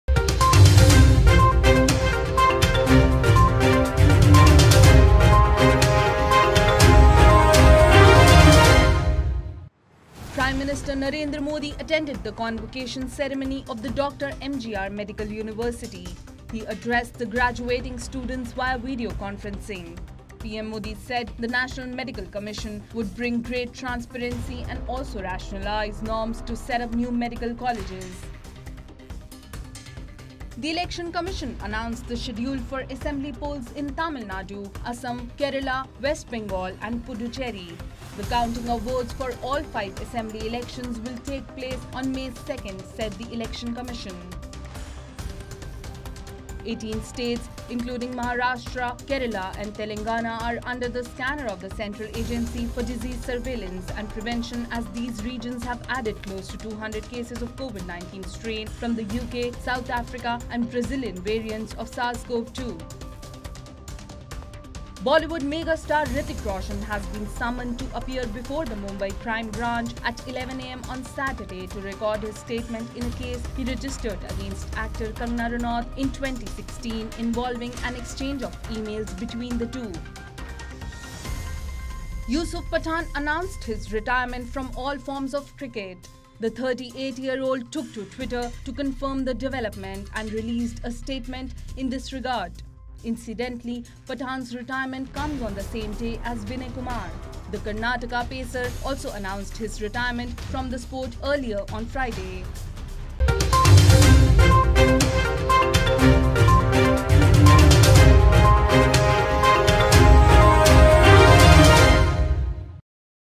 DNA: Top News of the Day | February 26, 2021 | Hrithik Roshan | Kangana Ranaut | Yusuf Pathan | Modi
Here's a roundup of top stories from the world of politics, business, world and entertainment, which grabbed the spotlight and trended the most on various social media platforms on February 26 (Friday).

In top news of the day, Prime Minister Narendra Modi attended the convocation ceremony of the Dr MGR Medical University. He addressed the graduating students via video conferencing. PM Modi said the National Medical Commission would bring great transparency and also rationalise norms to set up new medical colleges. In entertainment, Bollywood megastar Hrithik Roshan has been summoned to appear before the Mumbai crime branch at 11 am on Saturday to record his statement in a case he registered against actor Kangana Ranaut in 2016 involving an exchange of emails between the two. Meanwhile, in sports, Yusuf Pathan announced his retirement from all forms of cricket. The 38-year-old took to Twitter to confirm the development and released a statement in this regard. Incidentally, Pathan's retirement comes on the same day as Vinay Kumar. The Karnataka pacer also announced his retirement from the sport earlier on Friday.